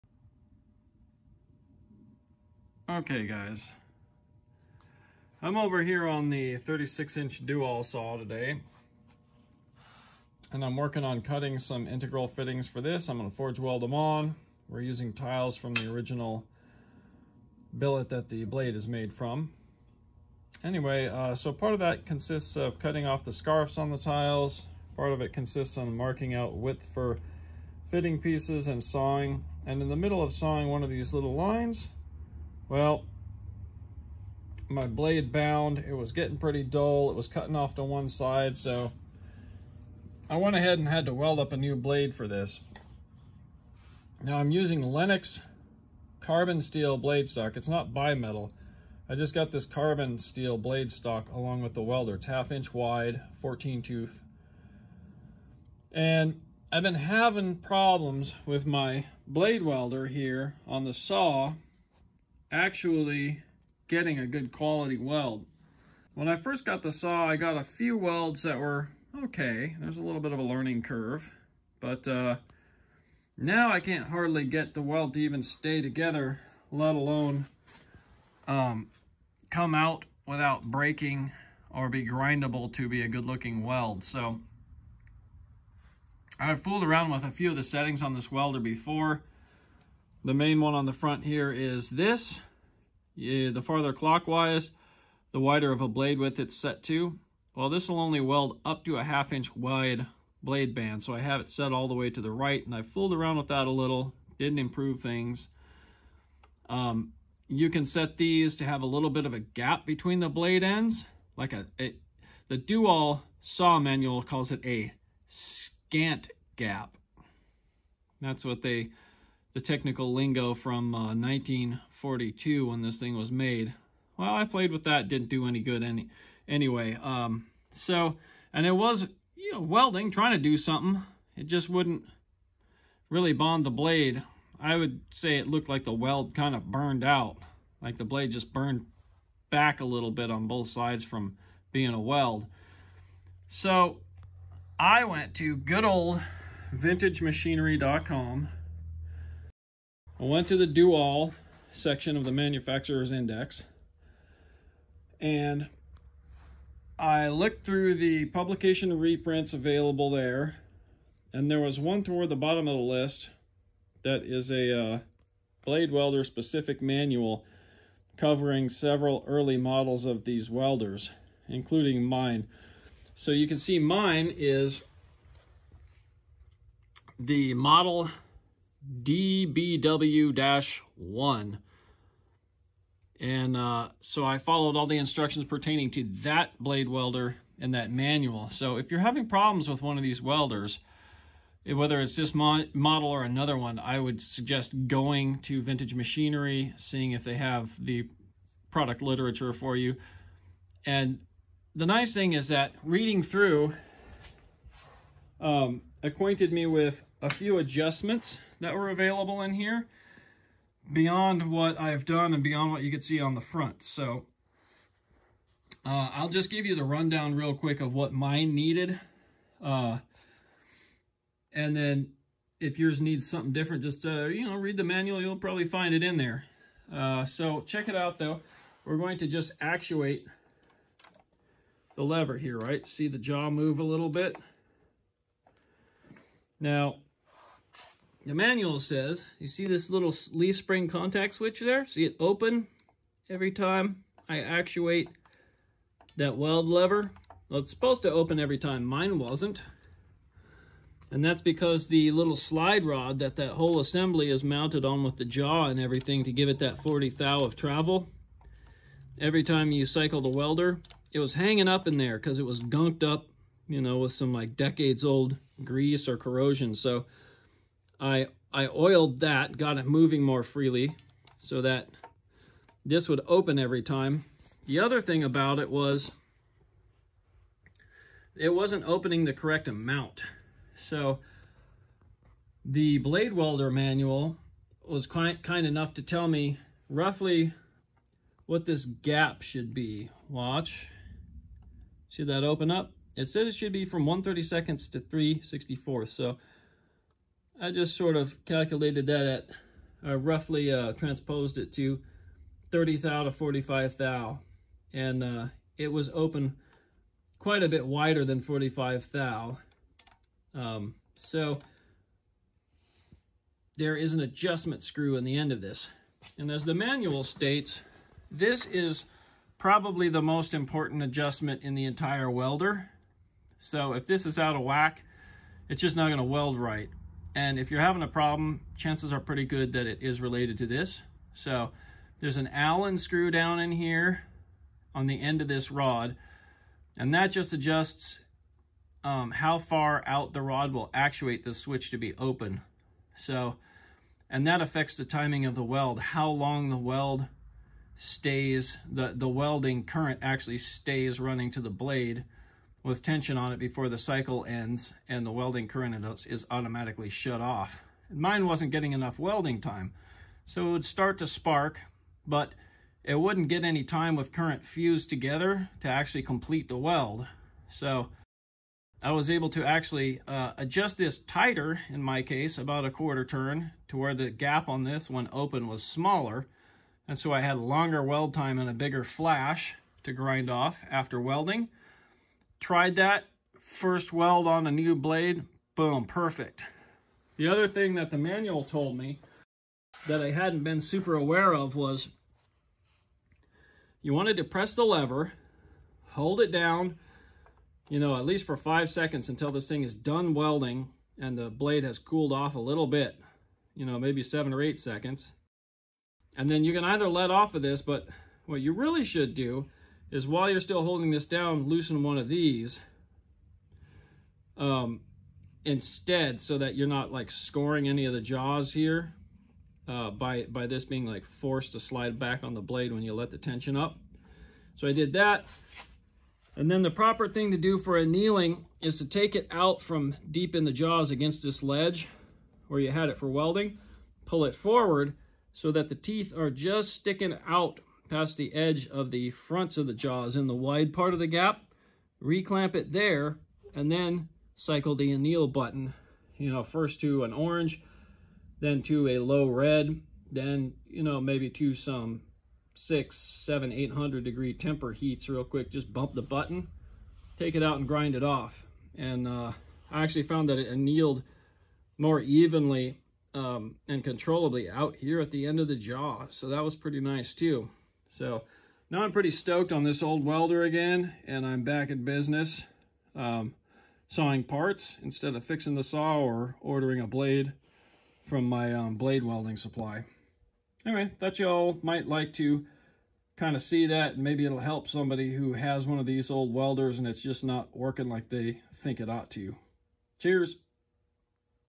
DoAll Bandsaw Blade Welder Fix!
In which I show some adjustments I found effective, in restoring my 1942 DoAll DBW-1 blade welding unit to optimum functionality.